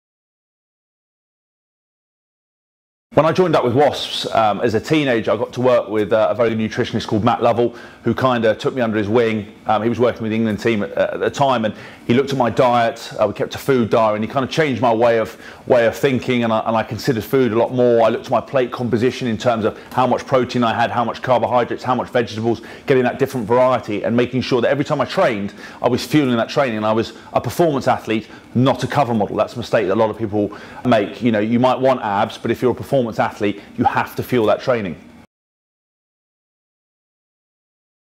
James Haskells talks training fuel with FFF
'If you're a performance athlete you have to fuel that training'.

Nutrition should be bespoke to you and your goals. That's why we work with you to create bespoke packages allowing you to perform at the top of your game. Food is fuel, and it's important we fuel our individual activity and goals rather that focusing on someone else's diet.

James talks to us about how he learnt to understand the way his plate fuelled his performance...